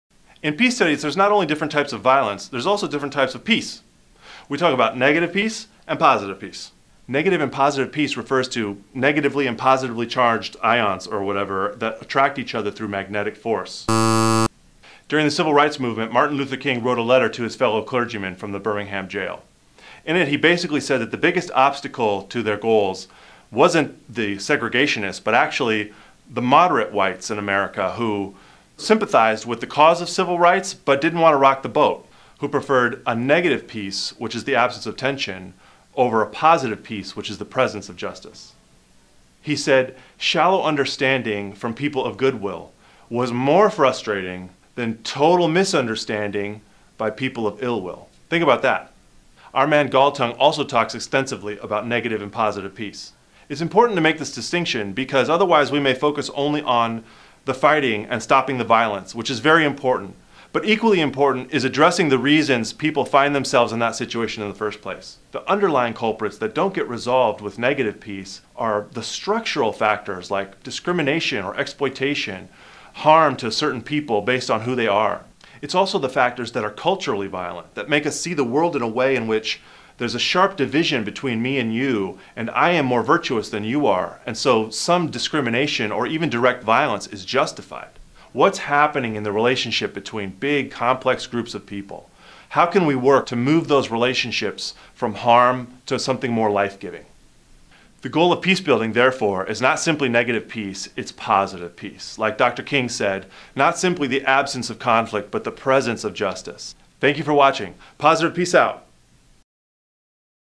Negative and Positive Peace - When is ending the fight not enough?
Negative peace means the absence of violence, which is a crucial and urgent goal for peacebuilders. But equally important is working for positive peace. It means changing the deeply-rooted factors that lead groups of people into conflict in the first place, such as feelings of exclusion, exploitation, injustice and histories of harm between different groups of people. We shouldn't settle for just removing something we don't want--we should also create something we DO want in its place.

Peace Studies instructor John Filson explains the difference between negative peace, which is the absence of violence, and positive peace, which is the presence of more just and beneficial relationships.